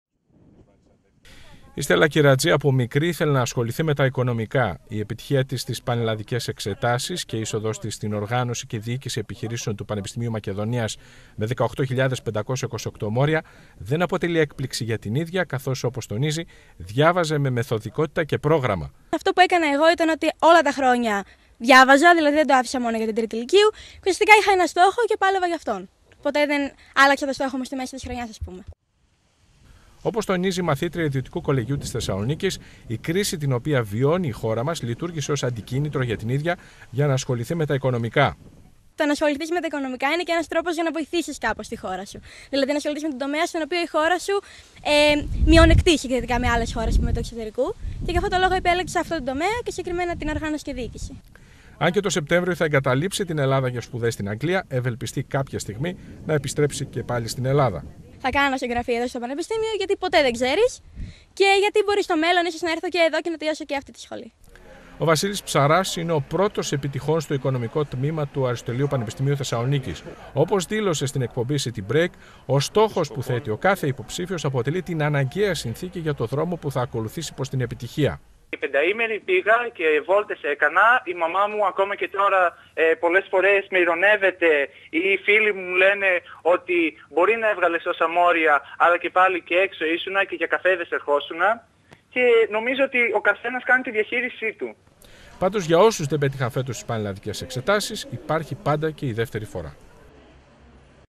ΑΡΙΣΤΟΥΧΟΙ ΒΑΣΕΙΣ 2016-ΠΡΩΤΙΕΣ ΠΑΝΕΛΛΑΔΙΚΩΝ (TV100-240816)
Η ανακοίνωση των βάσεων έφερε ανάμεικτα συναισθήματα στους υποψηφίους των πανελλαδικών. Άλλοι τα κατάφεραν, άλλοι πάλι όχι...Η Θεσσαλονίκη κατέγραψε και φέτος πρωτιές. Οι “πρώτοι των πρώτων”, παιδιά που πέτυχαν την εισαγωγή τους στα πανεπιστήμια με πολύ υψηλές βαθμολογίες μίλησαν στην TV100 για τον αγώνα που έδωσαν, για τις θυσίες που έκαναν, για τους στόχους και τα όνειρά τους...
Ρεπορτάζ: Γιώργος Κατσιάνης.
Κάμερα: Γιώργος Βογιατζόγλου.
Μοντάζ: Αλεξάνδρα Νικολούδη.
Μιλούν: Στέλλα Κυρατζή (Πρώτη Επιτυχούσα στην Οργάνωση και Διοίκηση Επιχειρήσεων ΠΑ.ΜΑΚ.), Βασίλης Ψαρράς (Πρώτος Επιτυχών στο Οικονομικό του ΑΠΘ)